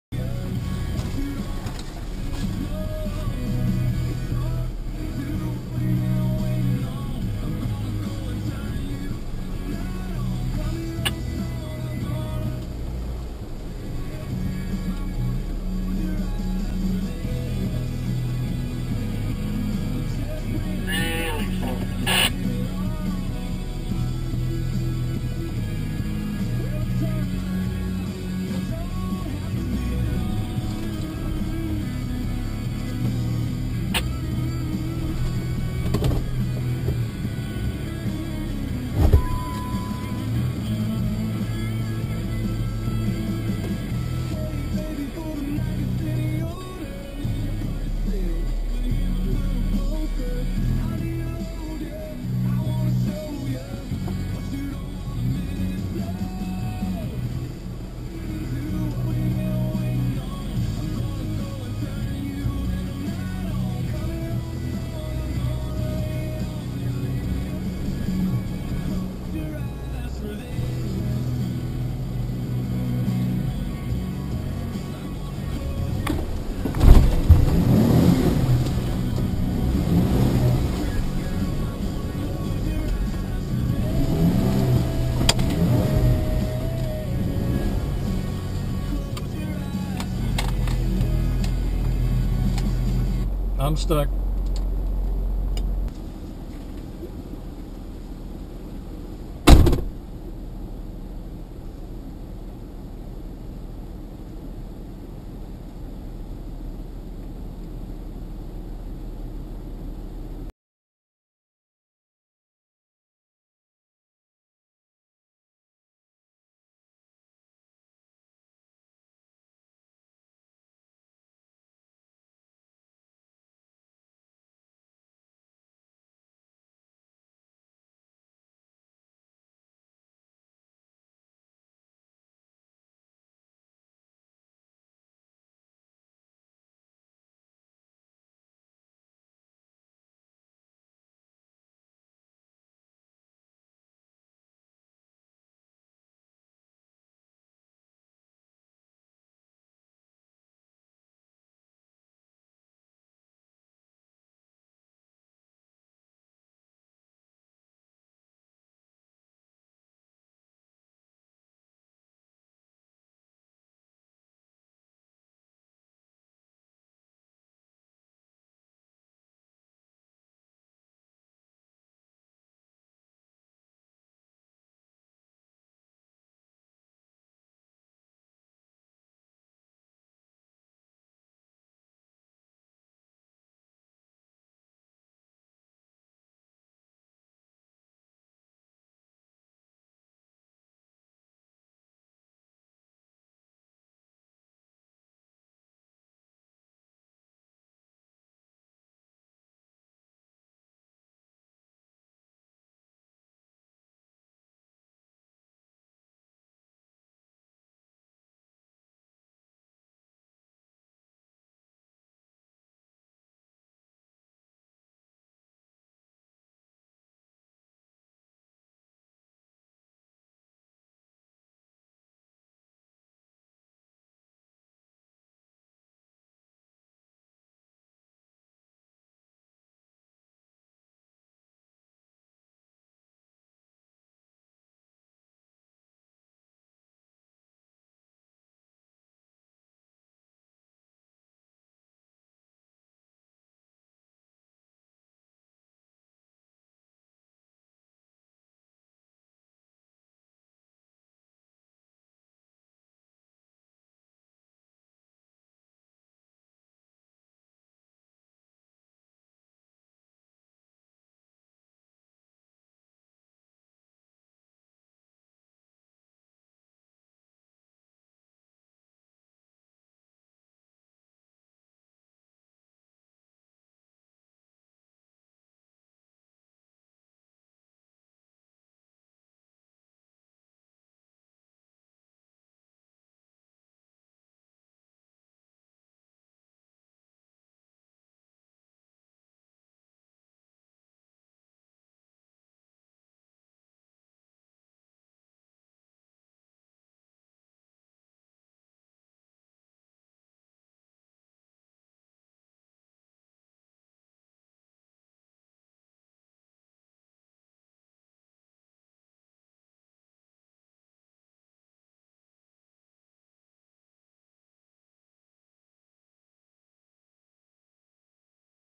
Log Problem
Trail riding in Lochloosa, FL. Didn't do so well traversing a sunken log.  Got to try out the winch and recovery gear. As you can see, a fast extraction.